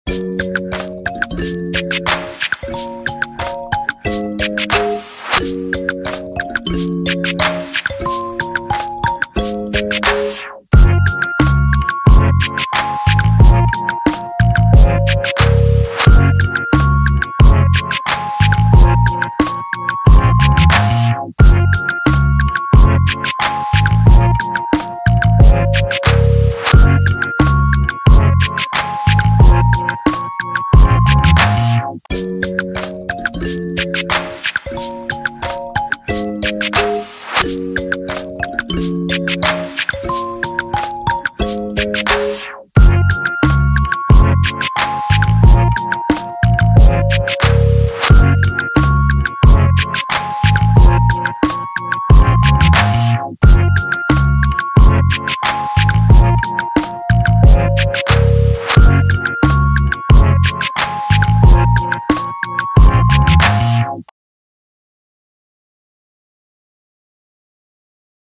True Triple Katana showcase!
In this video I tell you how to get TTK and demonstrate its abilities! enjoy!